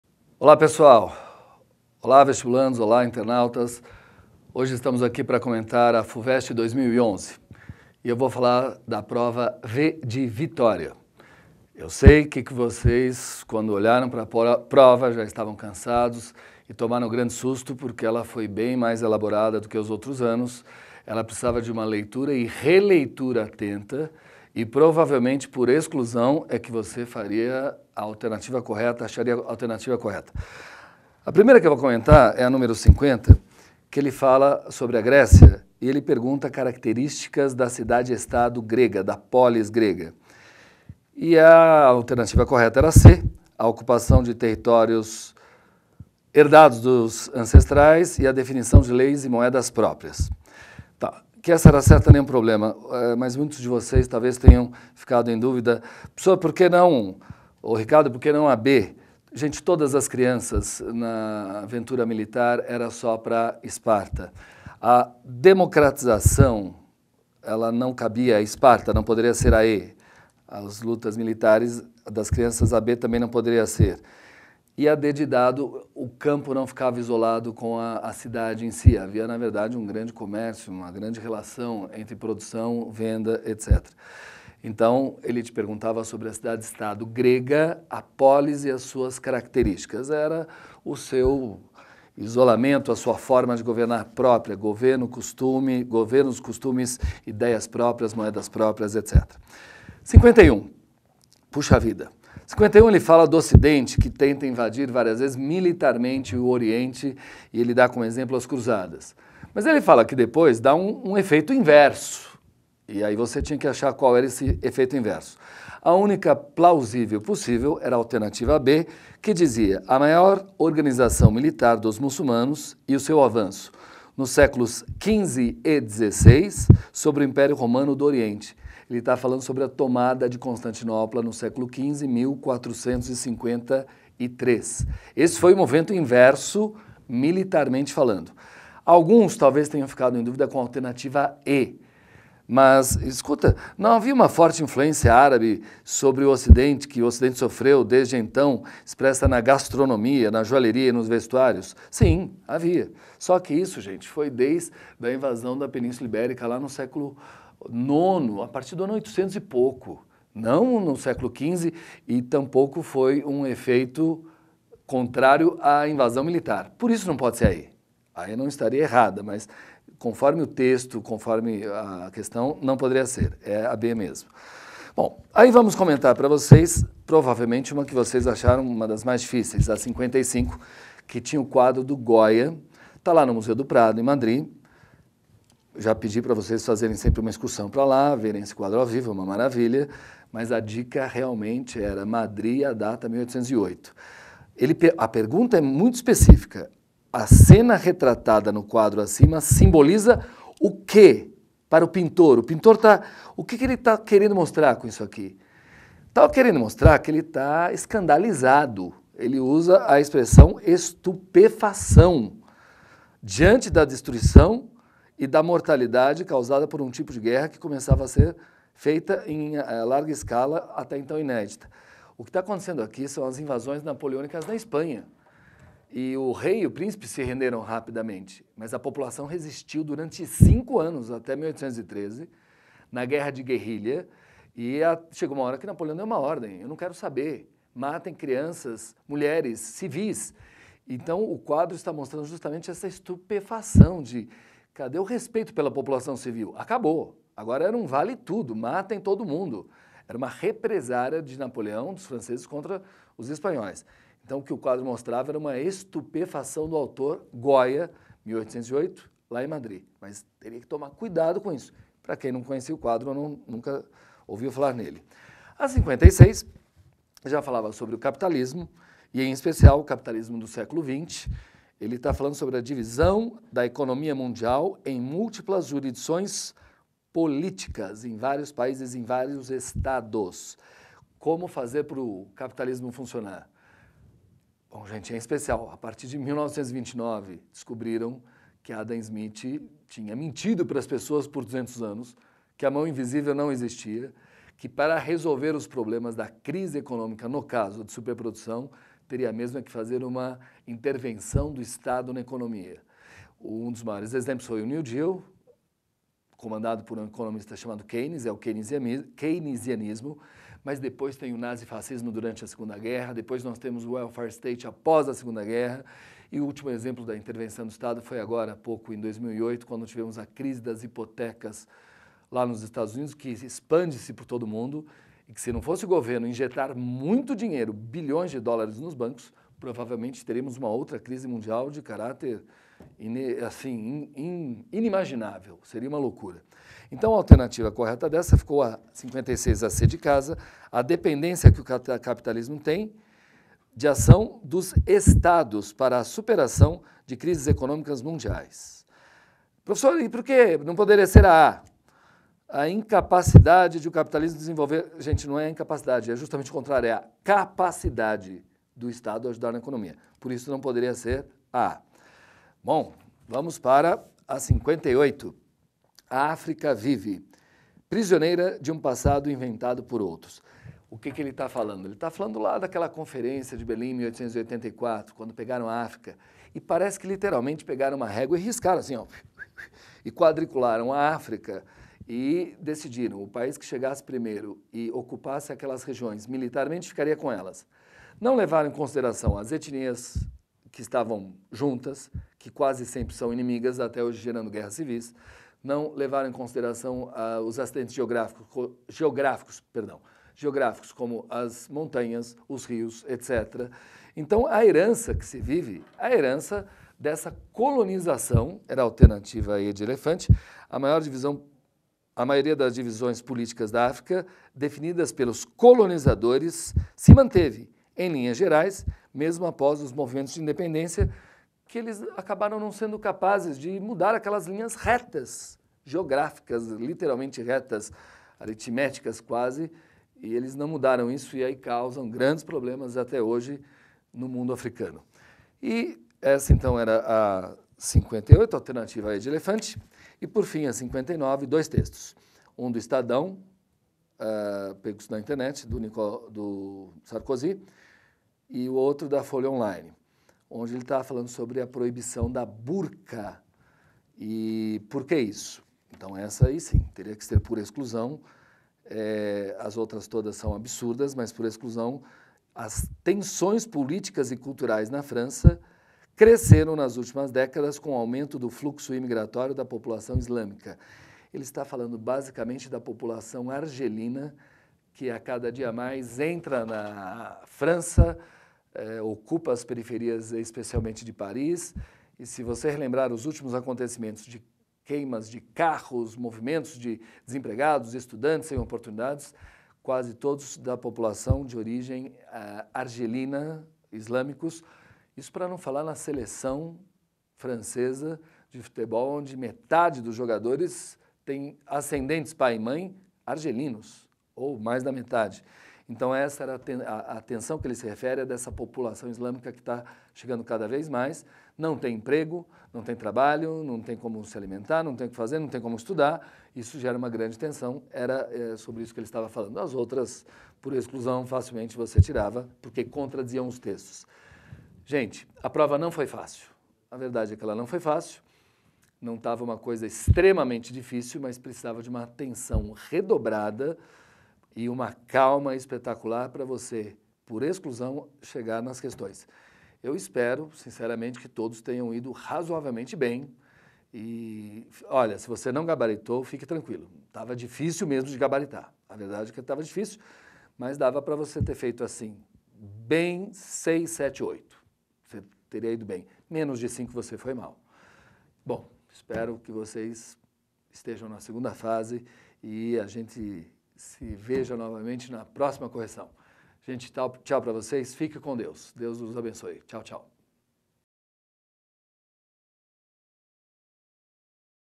1ª Fase História Geral - Fuvest 2011
Ricardo Palermo, professor de História do CPV Vestibulares, comenta o exame da Fuvest 2011 1ª Fase.